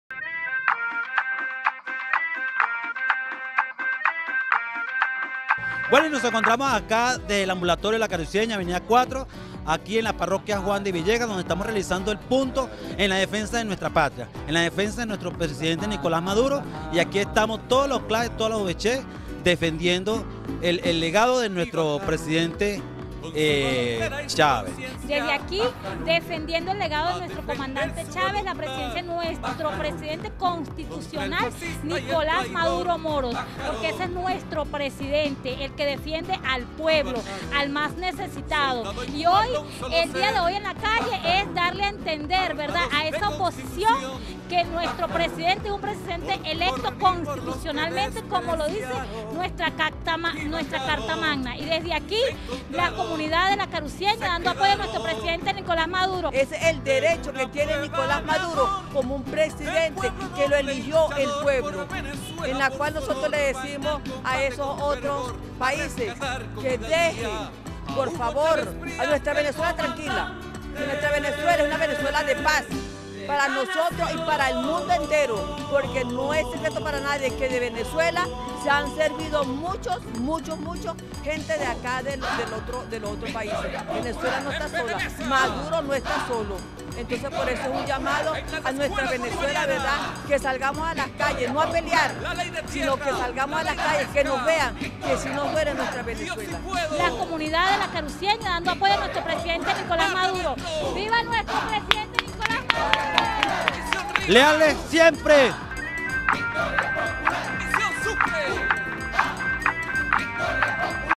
IC_Punto Rojo Antiperialista Carucieña
El pueblo Chavista Sale a la calle a ratificar su compromiso Con nuestro Presidente Nicolas Maduro y dar un rechaso a la intervencion de EEUU  en los asuntos de los Venezolanos.. el pueblo dice Leales Siempre Tridores Nunca...